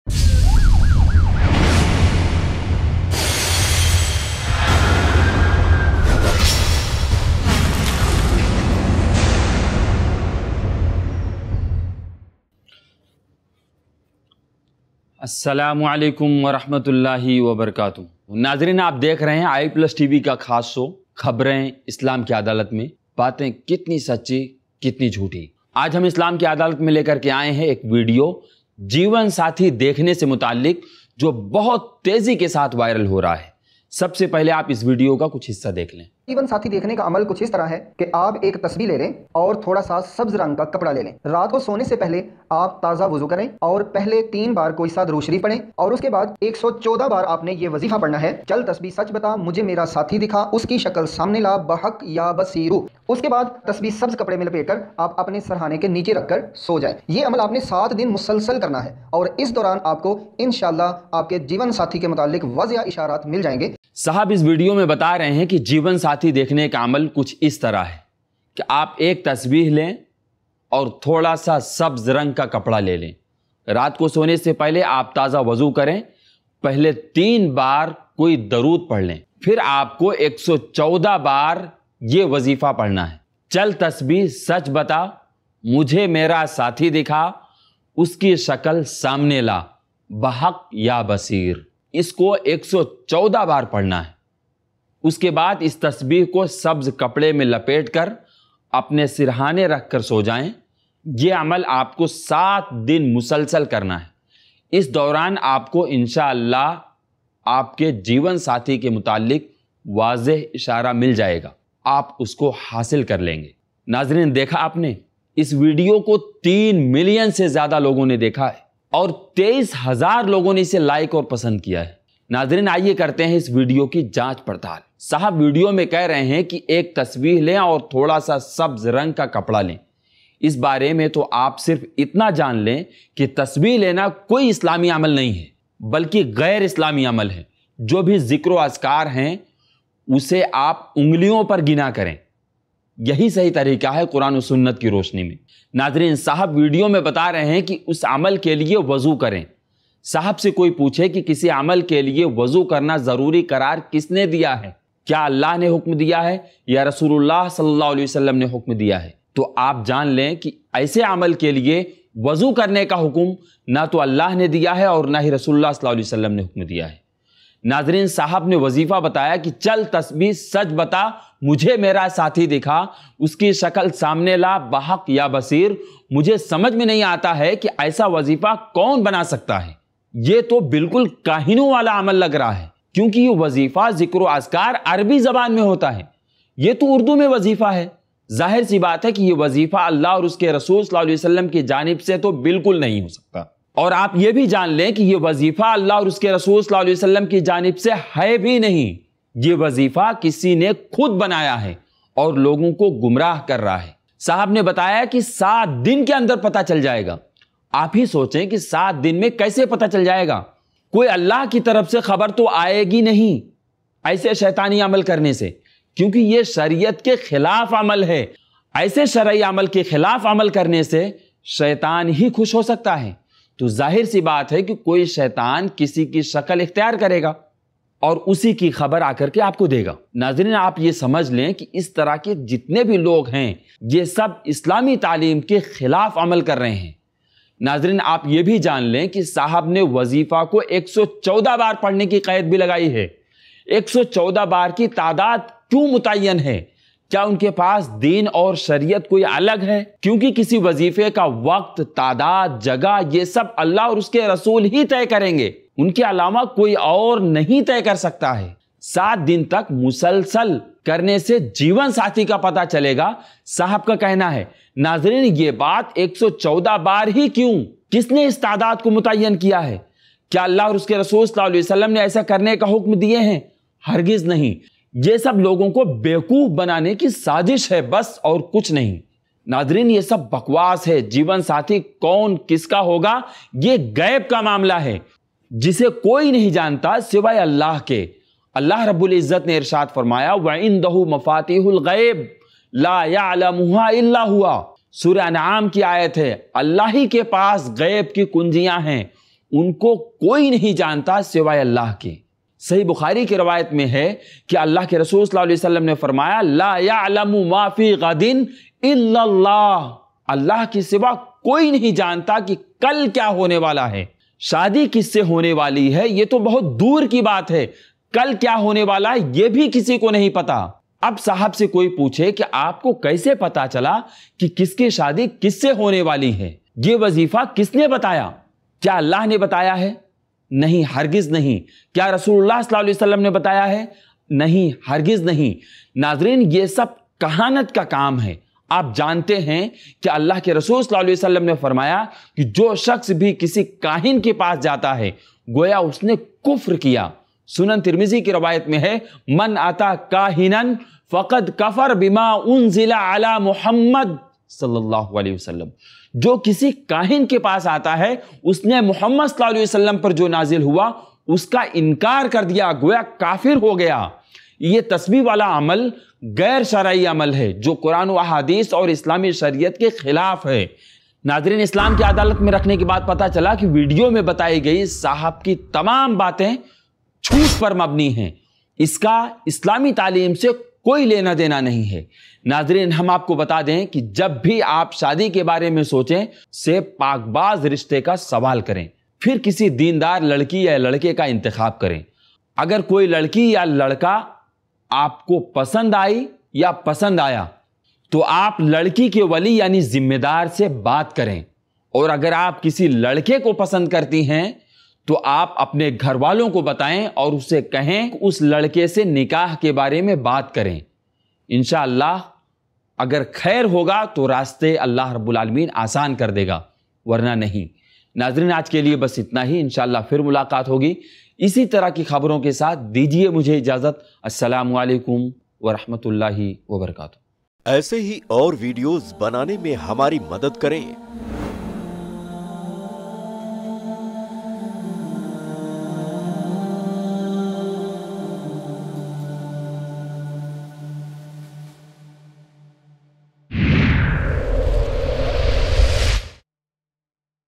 Jeevan Sathi Dekhne Ka Amal Ki Sachchai | Khabrein Islam Ki Adalat Mein Ep 44 | Sk. Hashim Daryabadi
Jeevan Sathi Dekhne Ka Amal Ki Sachchai | Khabrein Islam Ki Adalat Mein Ep 44 | Shaikh Hashim Daryabadi